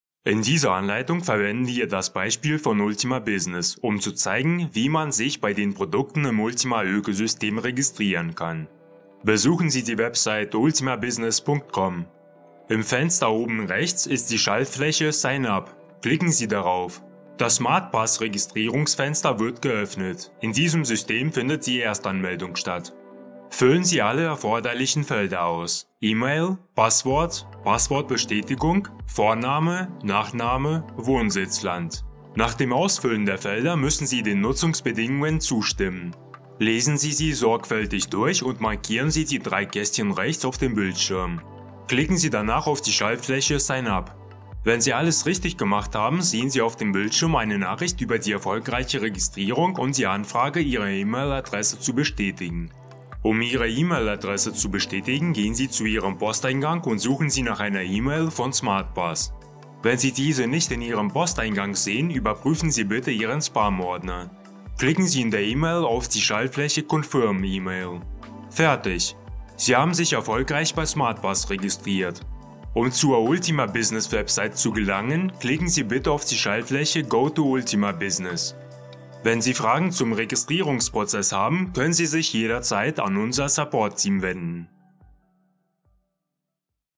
So registrieren Sie sich bei Ultima Business
☑️ Die Registrierung auf der Website Ultima Business ist der erste und wichtigste Schritt, der Ihnen Zugriff auf alle Produkte des Ökosystems Ultima eröffnet.In diesem Video haben wir den Registrierungsvorgang auf der Website ausführlich beschrieben, damit Sie alle Schritte schnell und problemlos ausführen können.

Einen Link zur Registrierung auf der Website können Sie von Ihrem Mentor oder Ihrem Partner erhalten.

Wenn Sie mehr über  ULTIMA BUSINESS erfahren möchten, folgen Sie uns in den sozialen Medien: